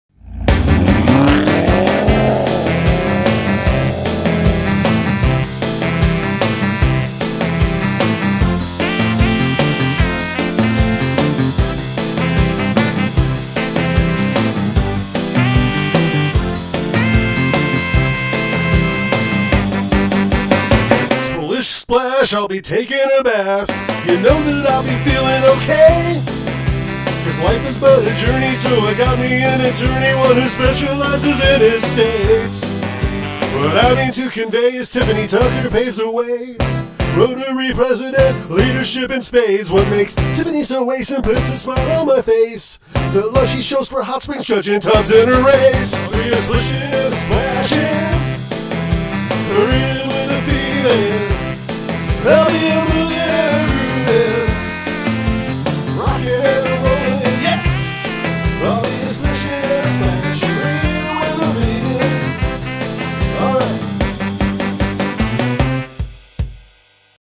"Hot Water Wheels" Bribe: Tiffany Tucker on behalf of Low Key Arts!
"Hot Water Wheels" will be representing Low Key Arts in this year's annual Stueart Pennington's World Championship Running of the Tubs June 4th.
While we gear up for the race, we hope you enjoy our bribe!